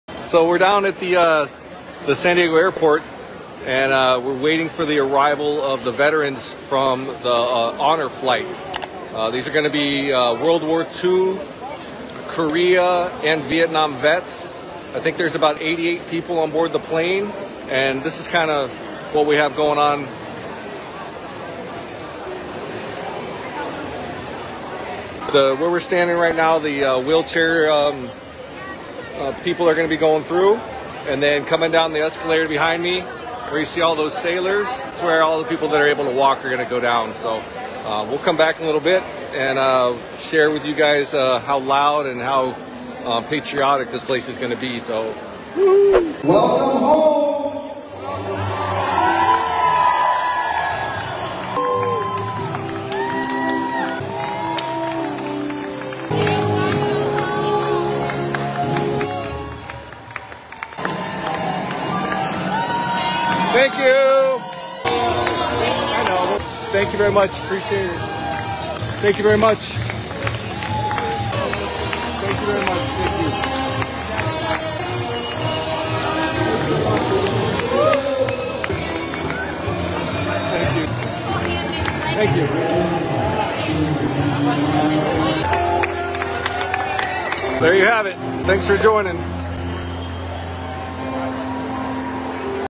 Bobster® Honor Flight In San Diego
Bobster® Heartwarming Honor Flight Experience in San Diego ✈️

We had the amazing chance to greet the veterans who took the Honor Flight and landed back here in San Diego.

What is this? Honor Flight is a remarkable initiative that gives veterans the opportunity to visit our nation's capital, Washington D.C., to pay tribute to their fallen comrades and witness the memorials dedicated to their sacrifices. It's a chance for these brave men and women to feel the nation's gratitude firsthand.